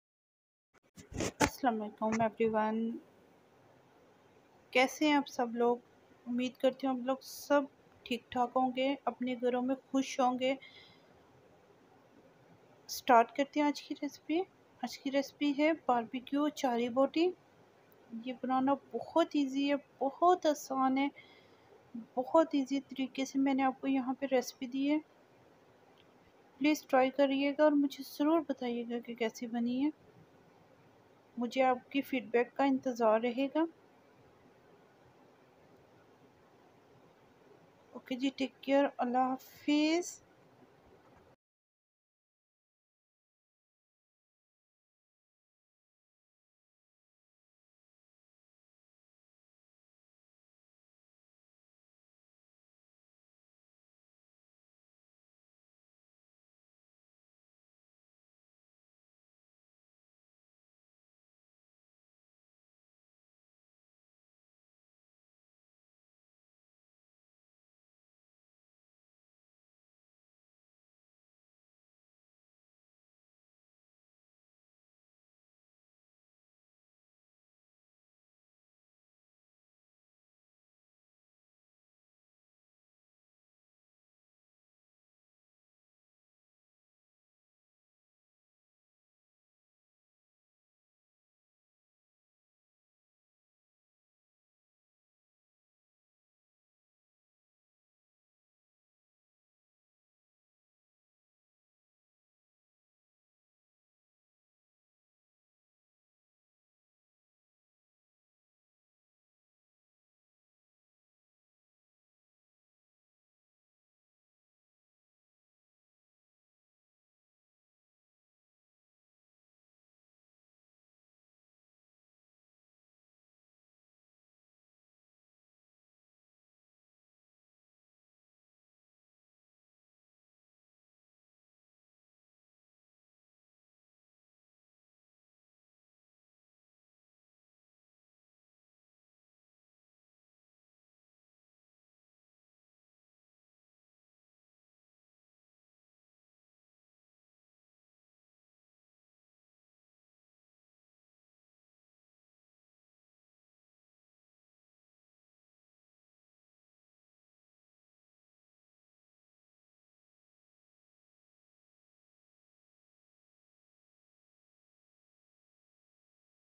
BBQ chicken achari Boti homemade Recipe..
Chicken
Thom
Black paper
Namak
Red mirch